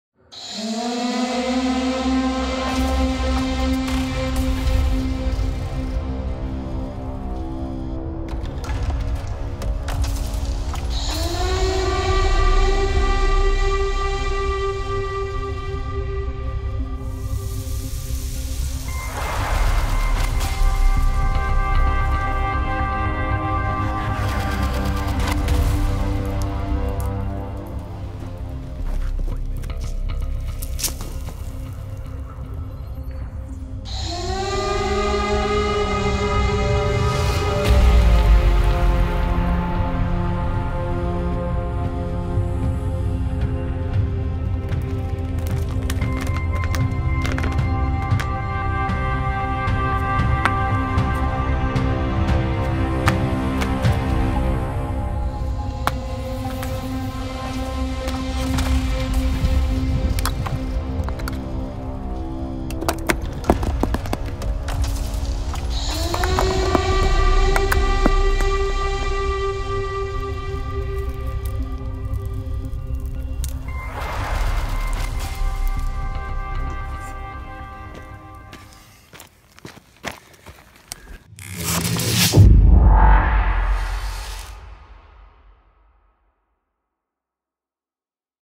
Recording Sound Effects at the Stone Pit
Impressions of our Stone Sound Effects Recording Trip to a Stone Pit...